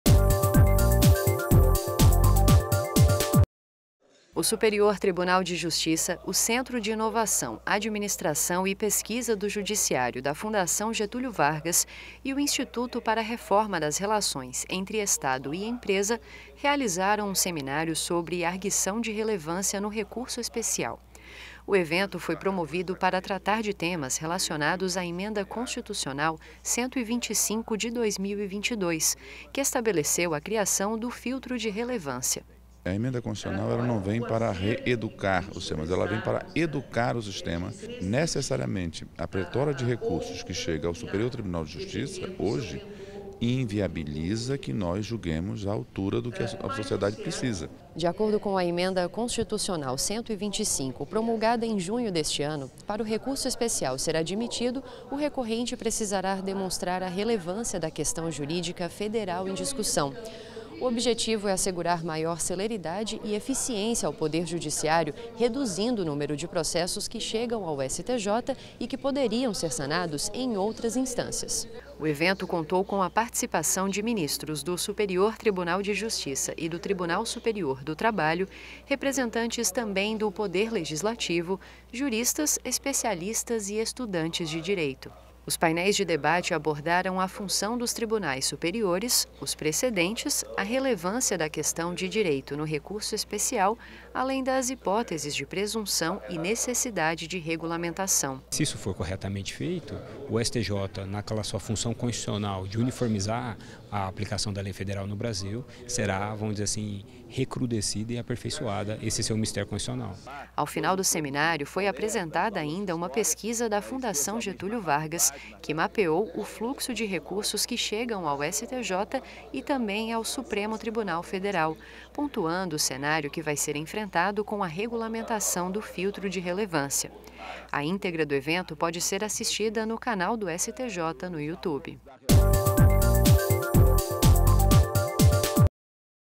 Seminário discute impactos do filtro de relevância no sistema de Justiça 28.09.22
O auditório do Superior Tribunal de Justiça (STJ) reuniu magistrados, advogados e outros especialistas, nesta quarta-feira (28), para uma discussão sobre o impacto esperado nas atividades da corte – e do Judiciário em geral – após a promulgação da Emenda Constitucional 125/2022, que cria o filtro de relevância para a admissão de recursos especiais.
Organizado pelo tribunal em parceria com o Instituto para Reforma das Relações entre Estado e Empresa (IREE) e o Centro de Inovação, Administração e Pesquisa do Judiciário da Fundação Getulio Vargas (CIAPJ/FGV), o seminário Arguição de Relevância no Recurso Especial foi coordenado pelo ministro do STJ Luis Felipe Salomão e pelos advogados Georges Abboud e Rodrigo Salomão.